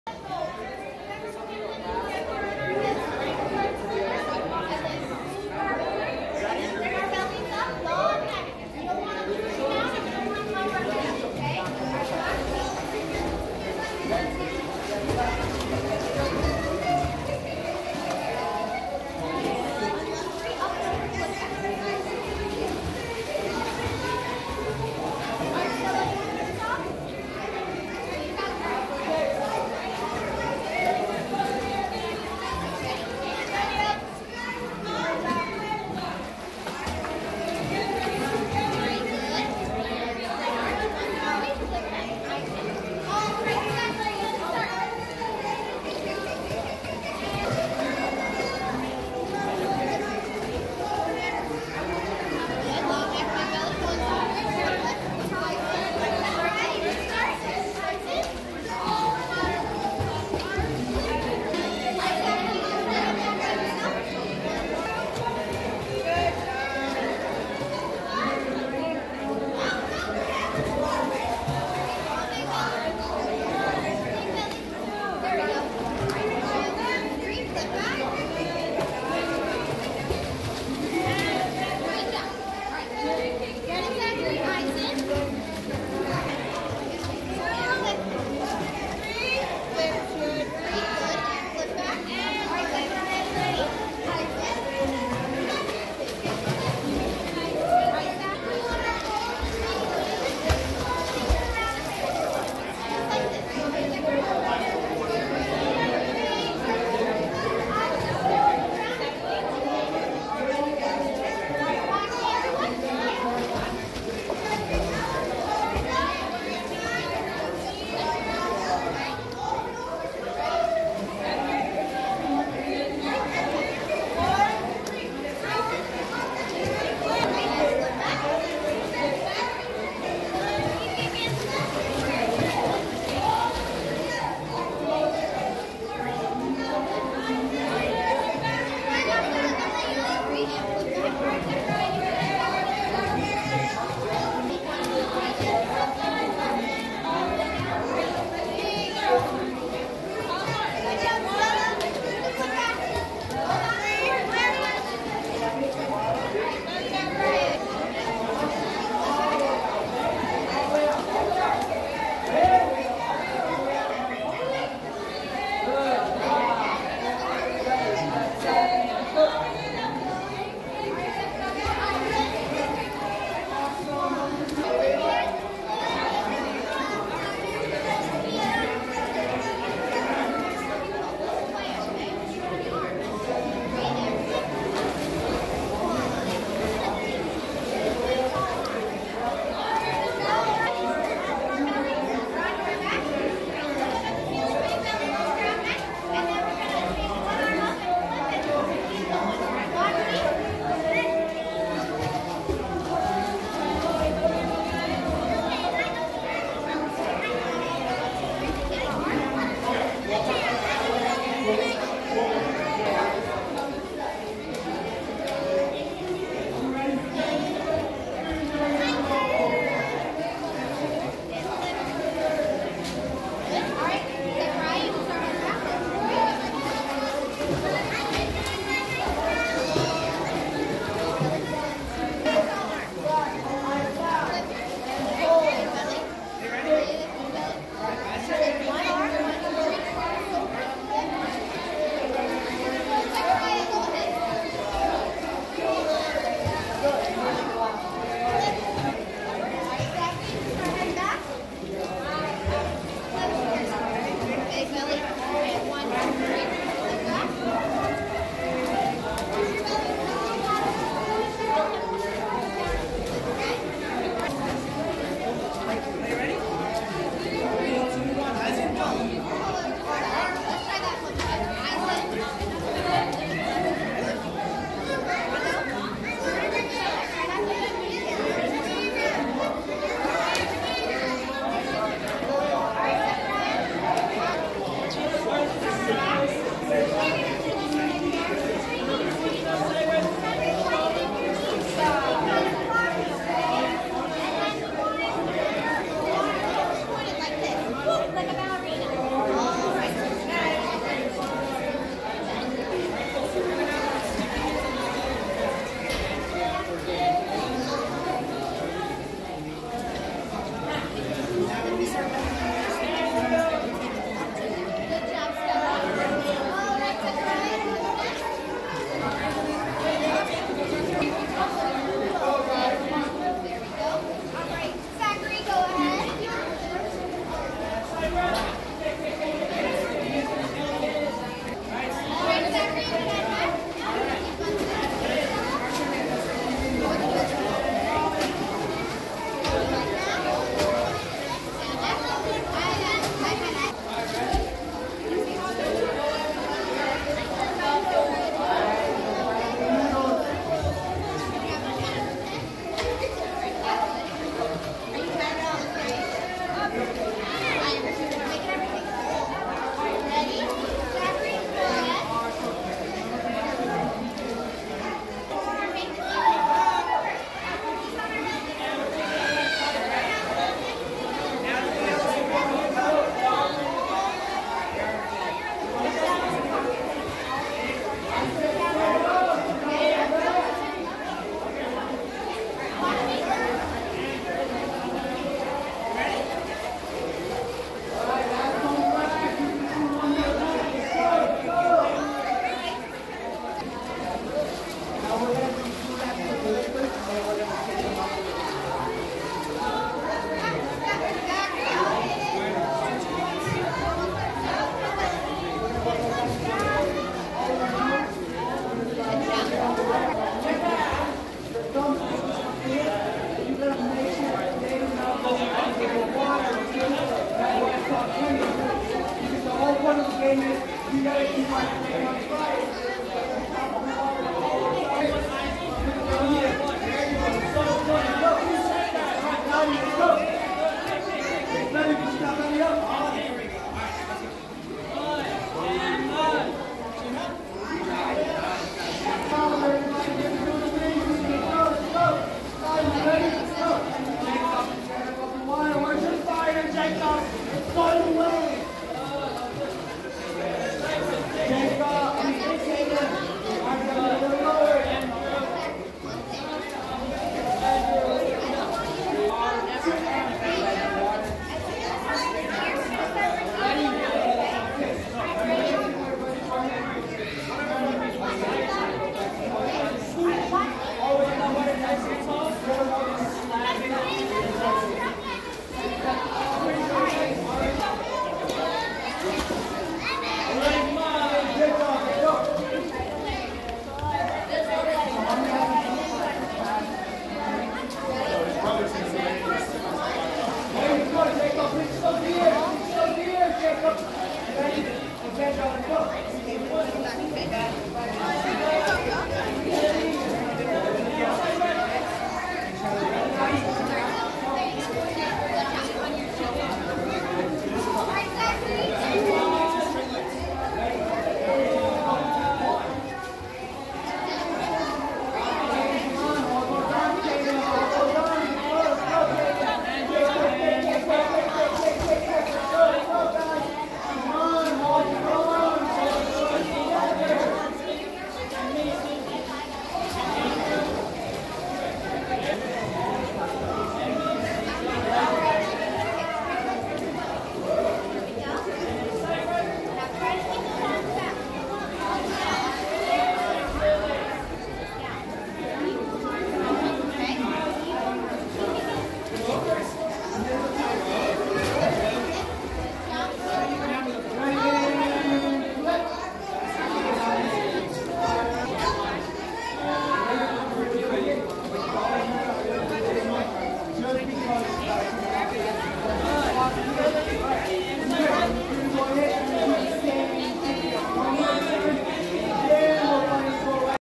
9 Minutes of Satisfying Water Splash Noises at Swimming Class Pool With Stella ASMR
9 Minutes of Satisfying Noises at New Swimming Class Pool With Stella ASMR